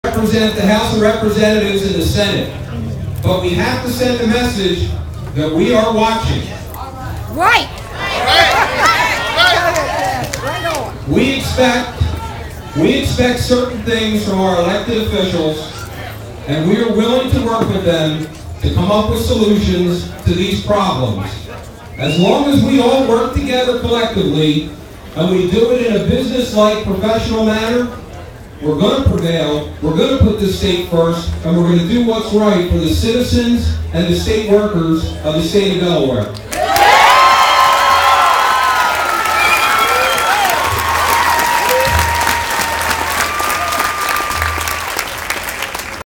Sgt. Vince Fiscella, Delaware State Troopers Association
Vince Fiscella is chair of "State Workers United for a Better Delaware," a coalition of 13 state employee unions which DSEA helped to form. United in its opposition to the 8% salary cut proposal, the coalition is asking the Governor and the General Assembly to find a fairer, more responsible way to help (not make worse) Delaware's economic future. Here Vince is greeting the crowd at our May 6 state employee rally attended by 1,000-2,000 state workers.